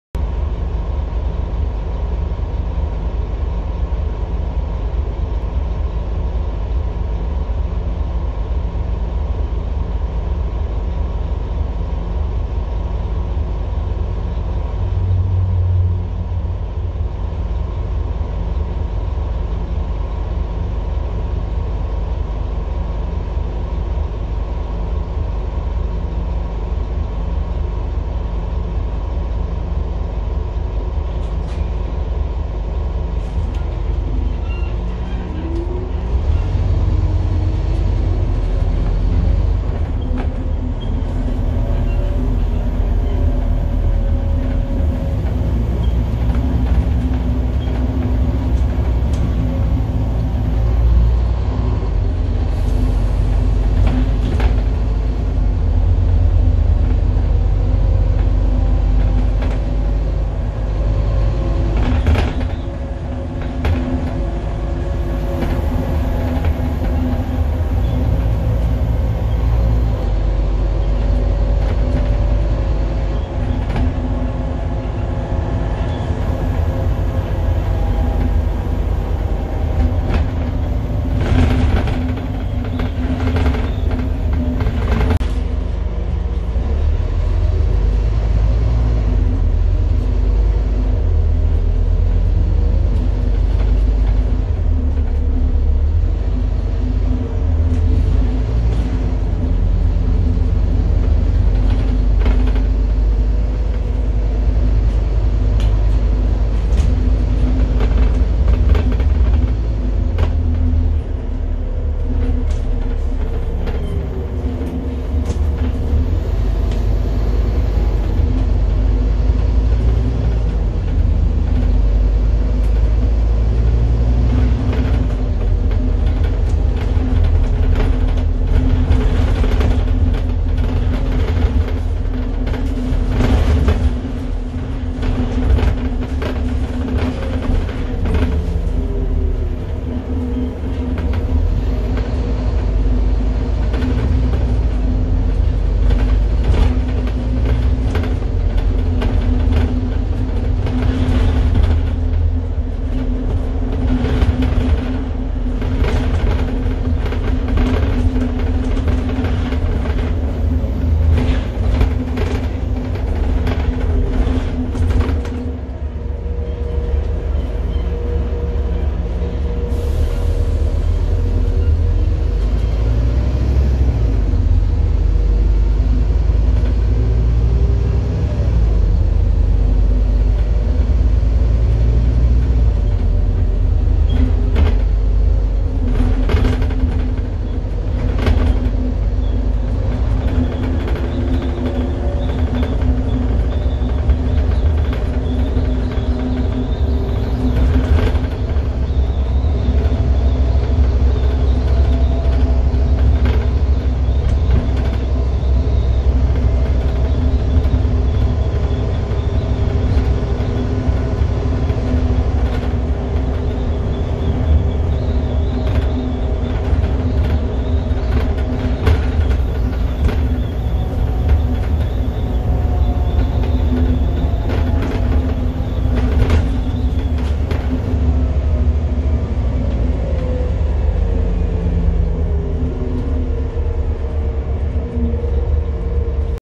Stagecoach Yorkshire 15590 GX10 HAO Scania N230UD Enviro 400
This vehicle came to Yorkshire in September 2020, but went for an Engine Rebuild and wasn't out for months until returning to service on June 11th 2021 which was when this was recorded.